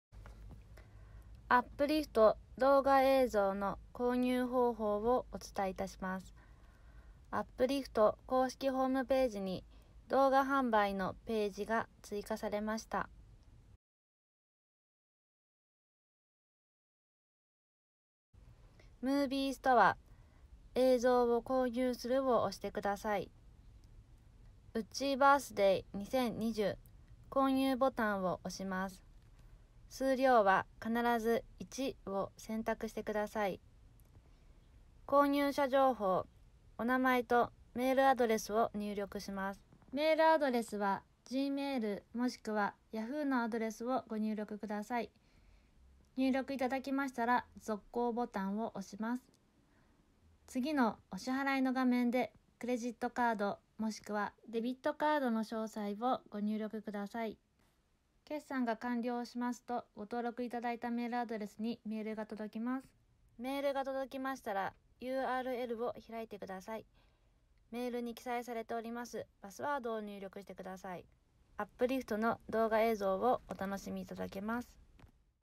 ホームページ動画購入の仕方
購入方法を説明します。

　【MOVIE STORE】の【映像を購入する】を選択

２、購入する動画の【購入ボタン】を選択
　　数量は、必ず【１】を選択後【お支払いへ進む】

３、購入者情報
　　名前・メールアドレスを入力後、【続行】を選択
　　※メールアドレスは、Gmail もしくYahooのアドレスを入力してください。

４、お支払い
　　クレジットカード・デビットカード情報を入力後
　　【購入する】を選択
　　決算が完了するとメールが届きます
　　※メールの再送信はできませんので、大切に保管をしてください

５、メールを開く
　　URLを開き、メールに記載されているパスワードを入力

６、動画が再生される
　　※繰り返し見ることができます